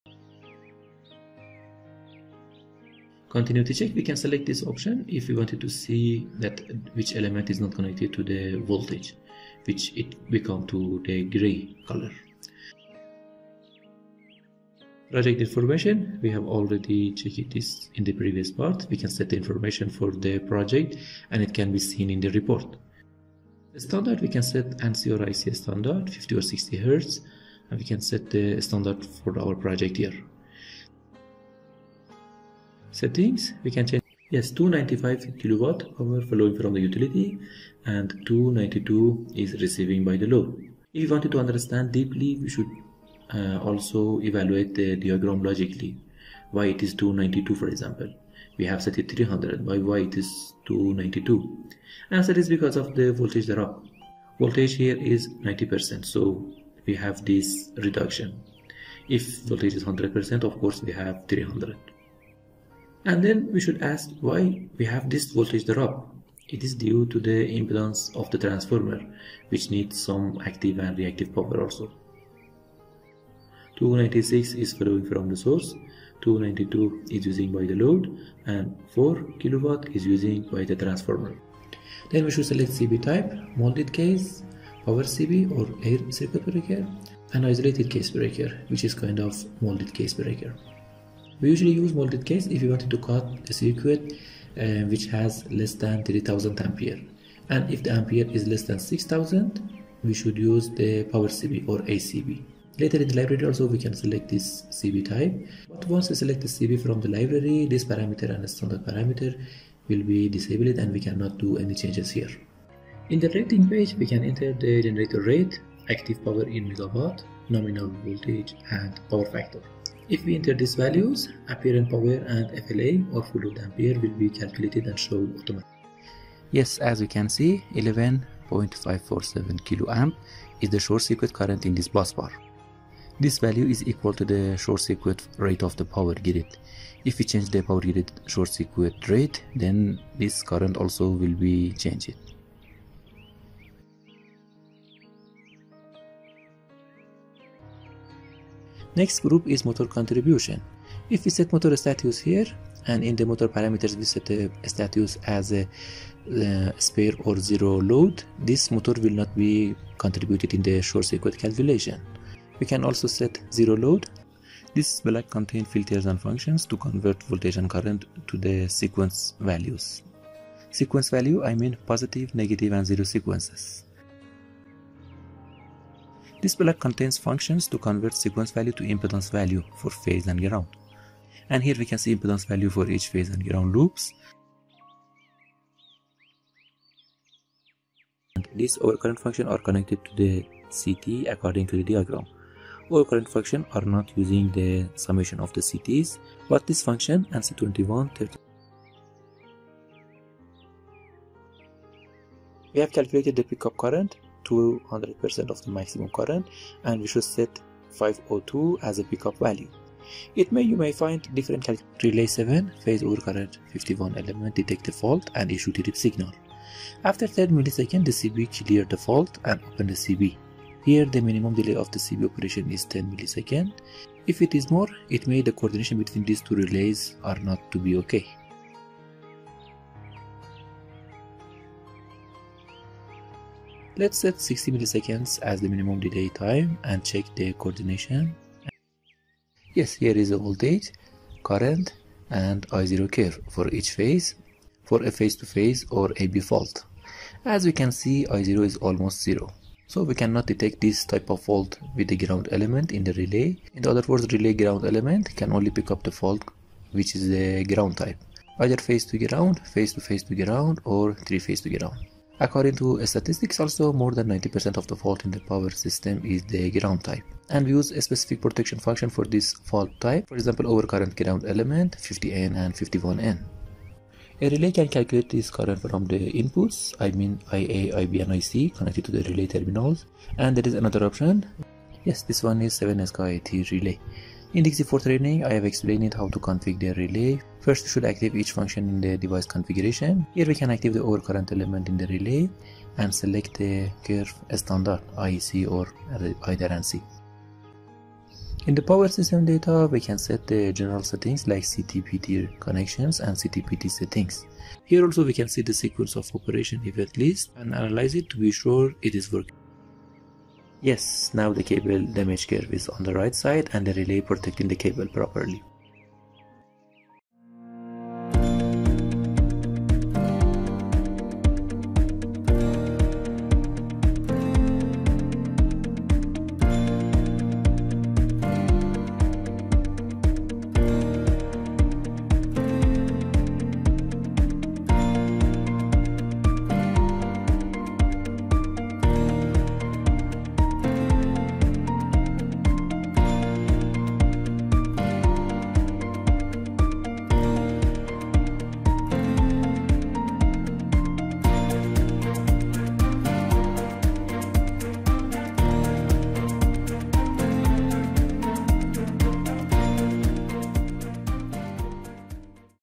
Protection and Coordination study with ETAP
ETAP is a very helpful software in the area of protection relay settings and coordination. We can analysis power system and calculate relay settings with this software.